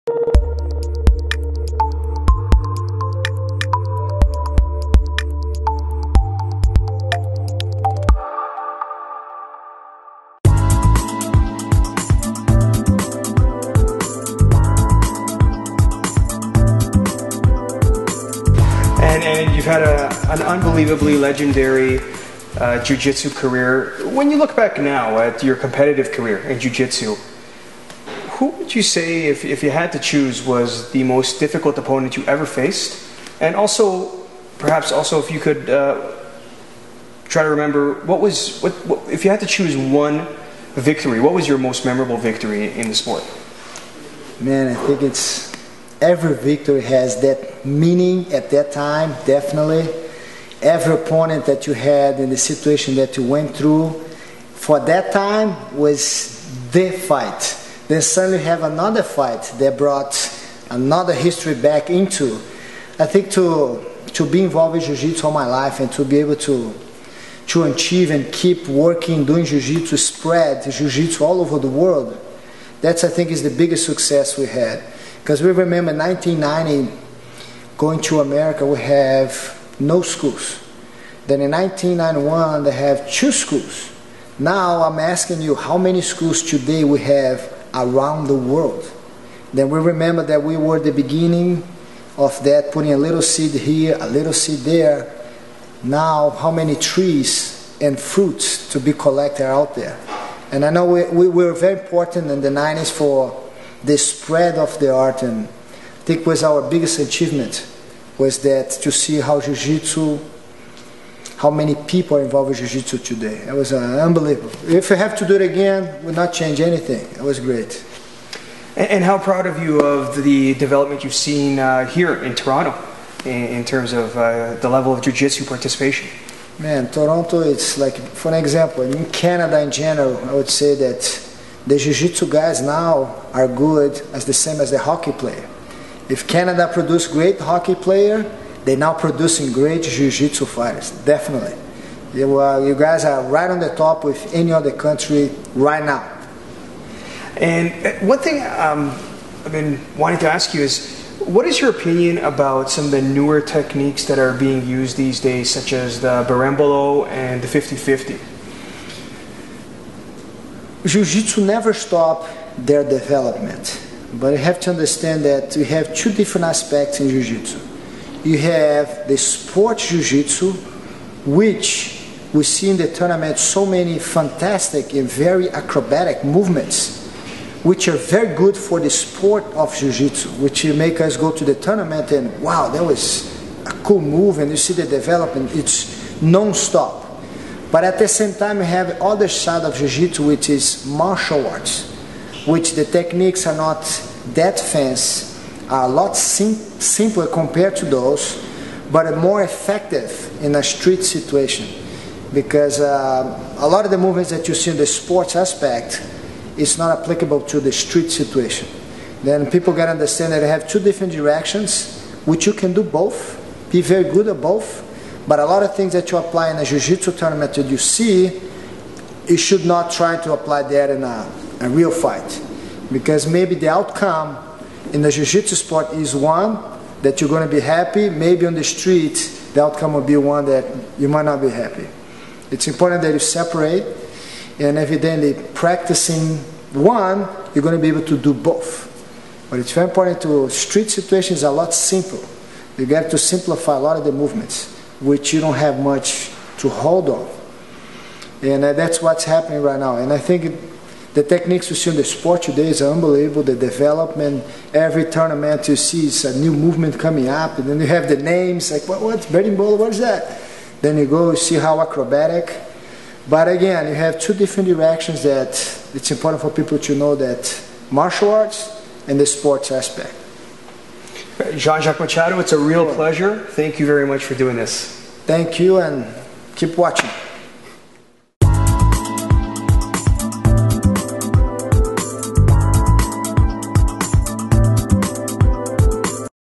Jean Jacques Machado Interview PART 2 - Jits Magazine
Jits Magazine's David Abbou talks with BJJ legend Jean Jacques Machado about a few of his philosophies and updates of late. In this part, JJ talks about his career, his opinion on the BJJ scene in Canada and the importance of keeping sport and combat jiu-jitsu separated. PART 2 of 2.